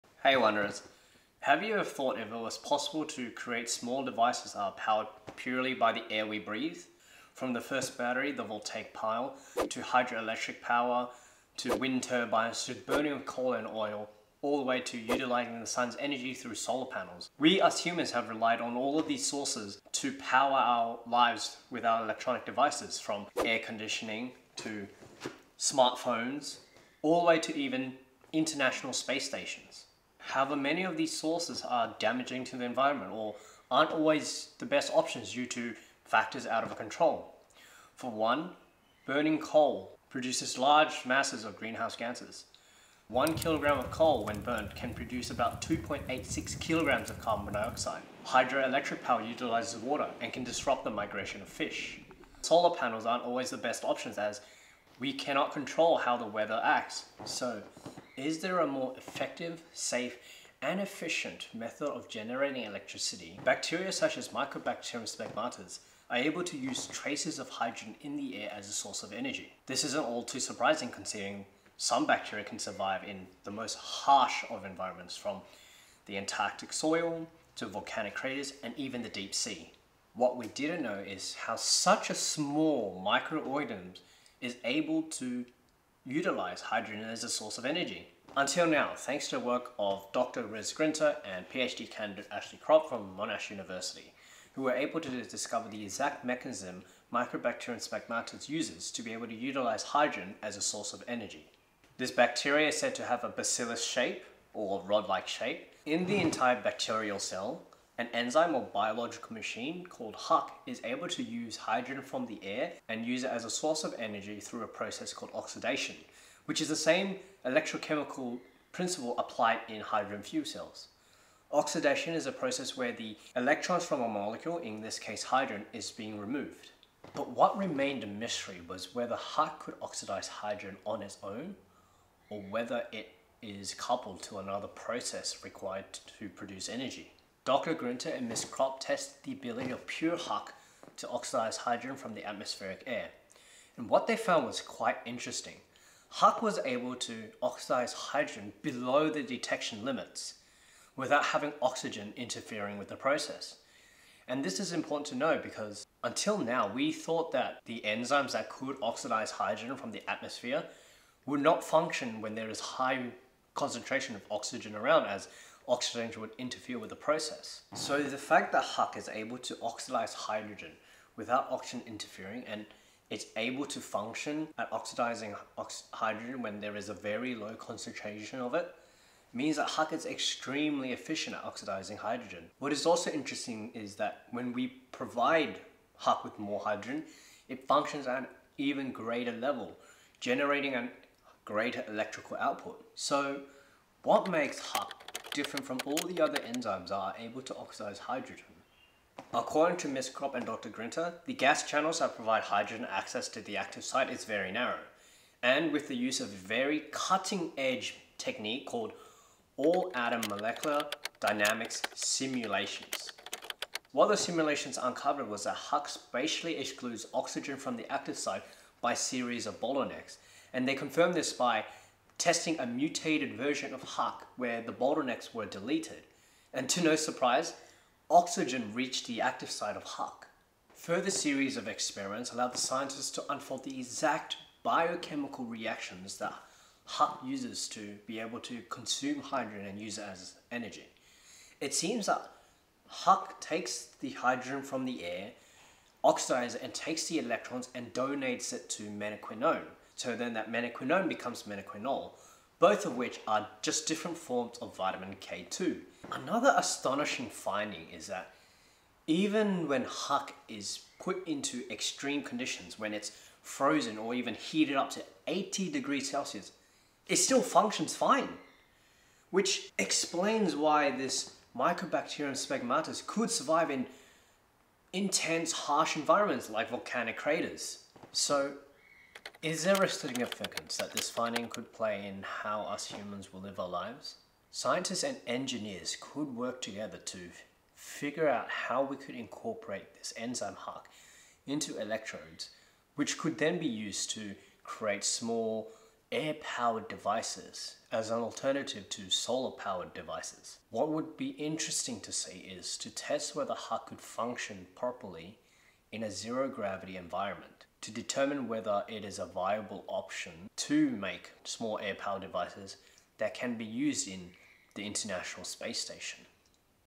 Bacteria turns Air into Electricity | Air-powered Devices?
Decades we have burnt coal and oil to generate electricity, however, these have negative effects. A new study found an enzyme inside a bacteria that turns hydrogen from the atmosphere into an electrical current. This could be a new way to produce eco-friendly energy.

References:
Grinter, R., Kropp, A., Venugopal, H. et al. Structural basis for bacterial energy extraction from atmospheric hydrogen. Nature 615, 541–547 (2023).